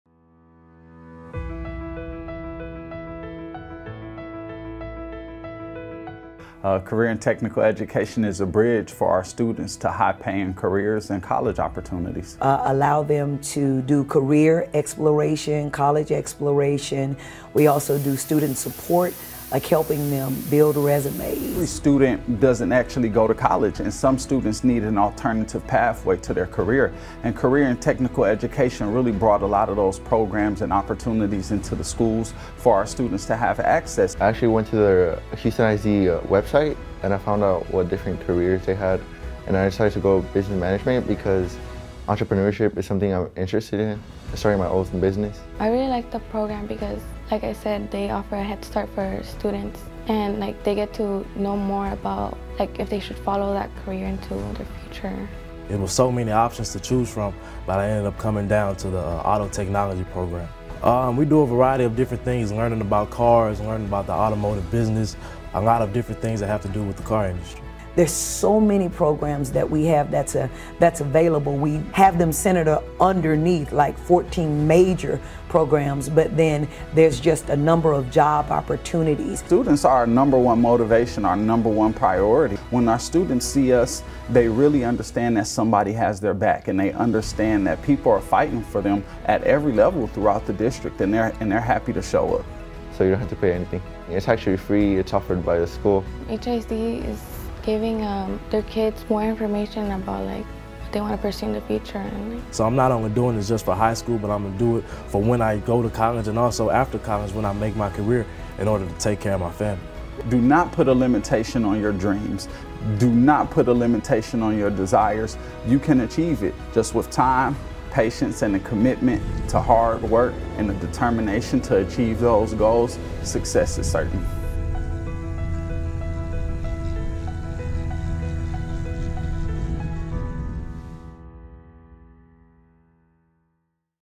Get To Know - HISD CTE Programs
“Career and technical education is a bridge for our students to high paying careers and college opportunities”

Watch this video to see how you could benefit from being a part of the HISD CTE Program :)